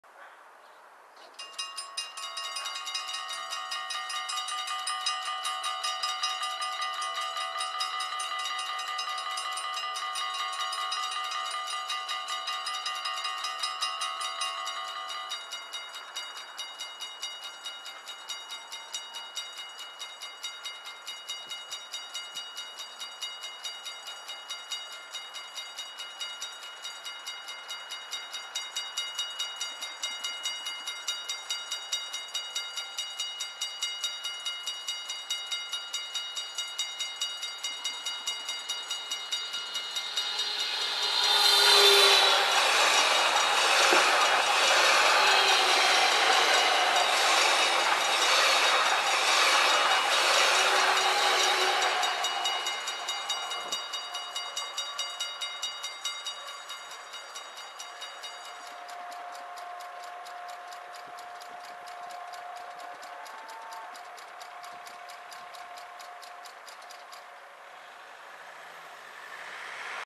Spoorwegovergang Venlo/ Passage a Niveau/ Railroad-/ Level Crossing/ Bahnübergang
De Stoptrein naar Venlo passeert de Bevrijdingsweg

Le Train omnibus a Venlo  passe un passage a niveau de Bevrijdingsweg

The Regional Rail to Venlo passes the Bevrijdingsweg

Das Nahverkehrszug nach Venlo fahrt durch die Bevrijdingsweg

d.d. 5 februari 2015